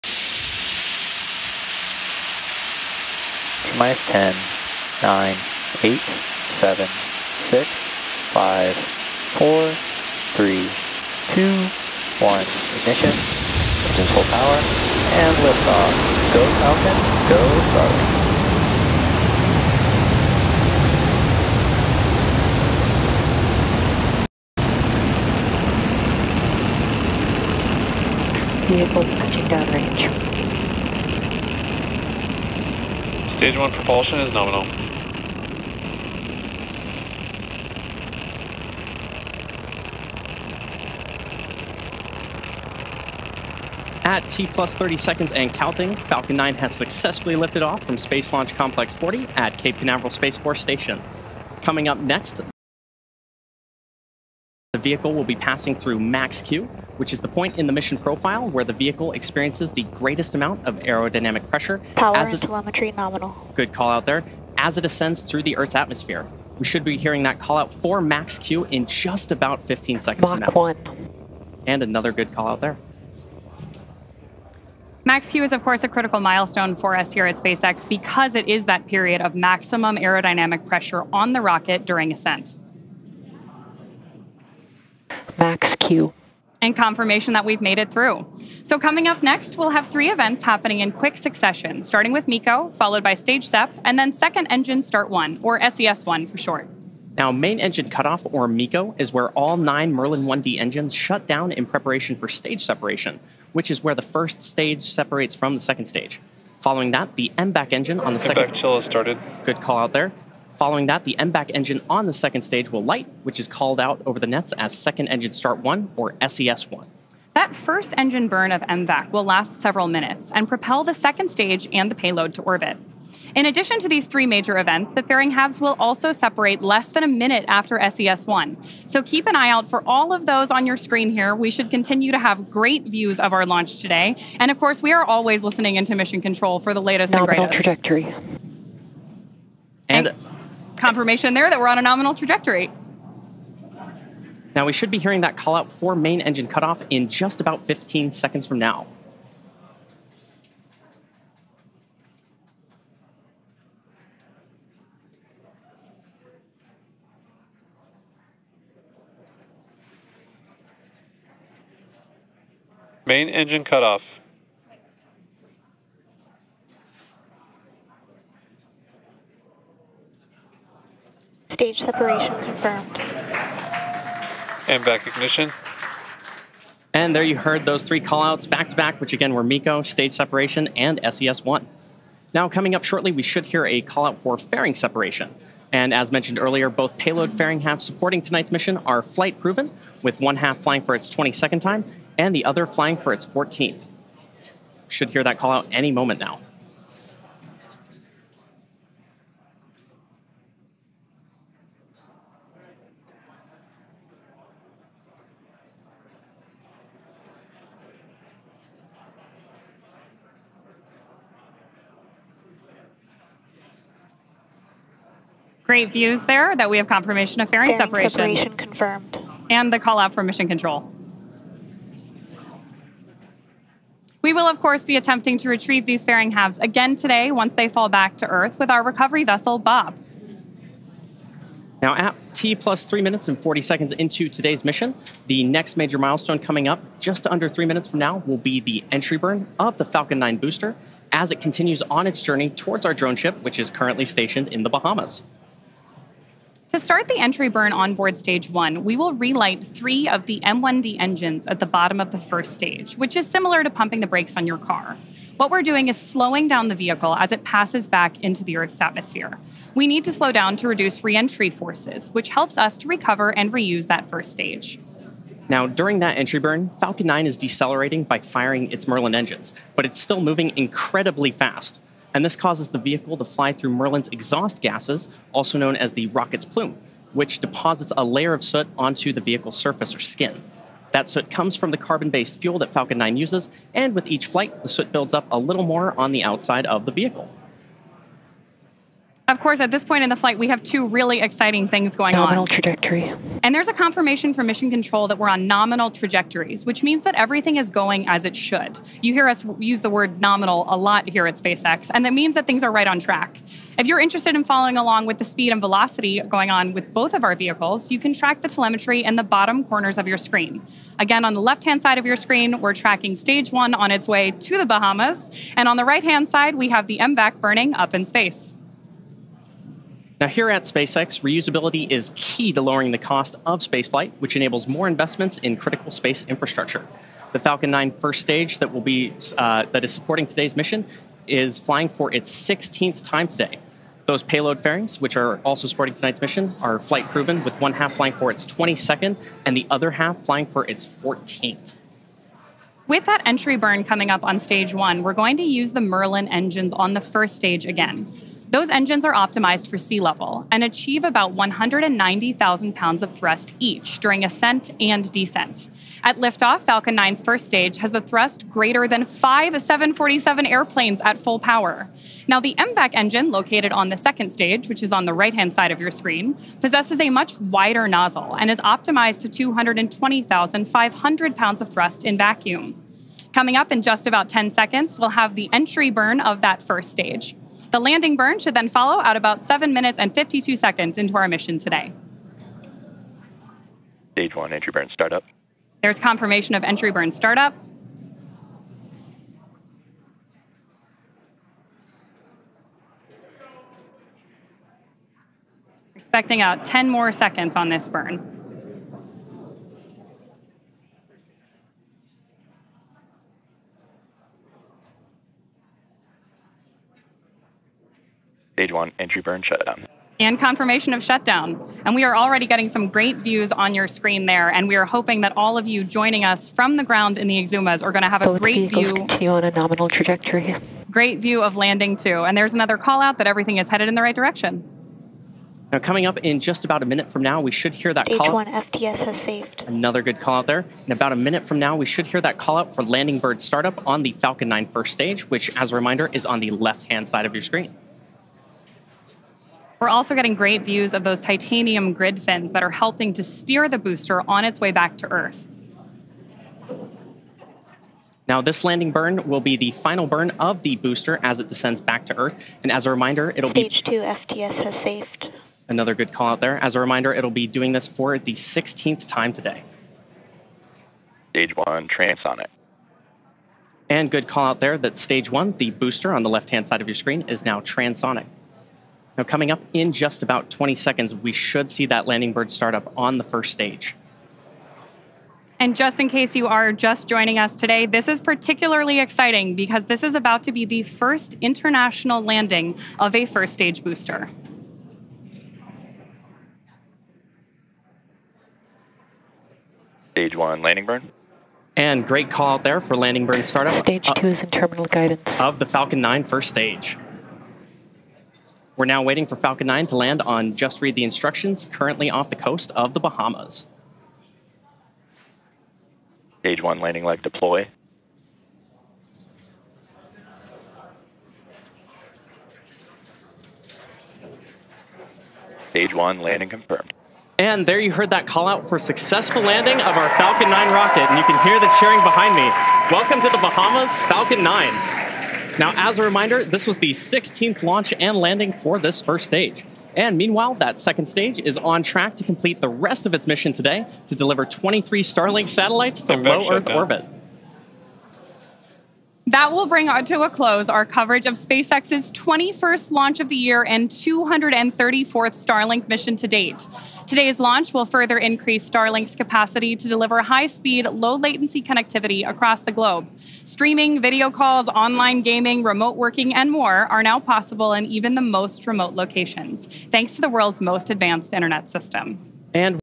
Blastoff! SpaceX launches Starlink batch, nails first landing off coast of The Bahamas
A SpaceX Falcon 9 rocket launched 23 Starlink satellites from Florida's Cape Canaveral Space Force Station on Feb. 18, 2025 at 6:15 p.m. EST (2315 GMT).

The rocket's first stage will came back to Earth about eight minutes after liftoff, landing on a drone ship stationed in the Atlantic Ocean off the coast of The Bahamas for the first time.

Credit: SpaceX